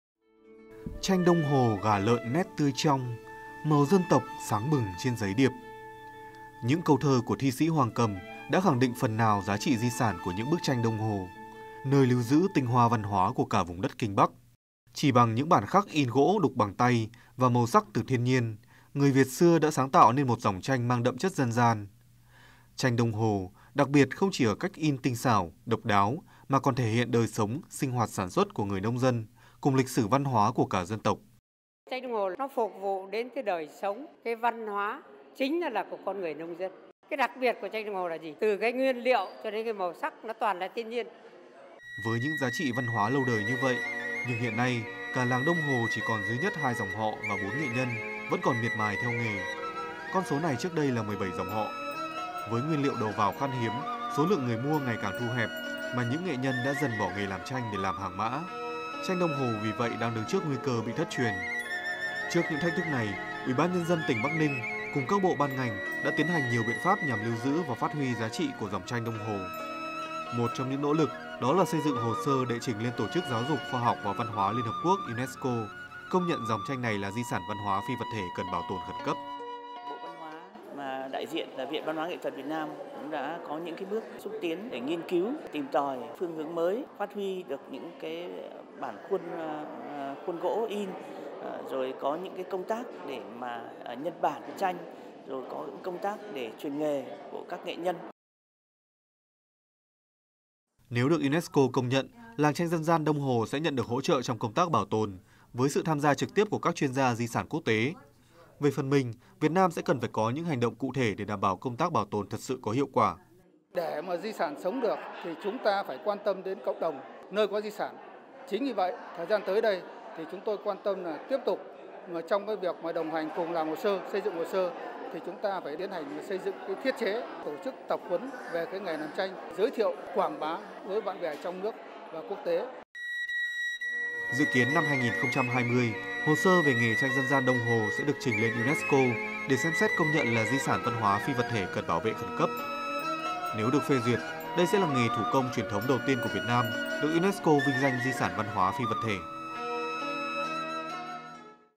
Dong Ho Woodblock Printing Faces Decline | VTV World
Dong ho woodblock printing was created by people in Dong Ho Village, Song Ho Commune, Bac Ninh Province. During the Lunar Tet holidays, Vietnamese people buy Dong Ho paintings for worship and house decoration since the prints represent good omens, luck, happiness, and depict the beauty of nature and country.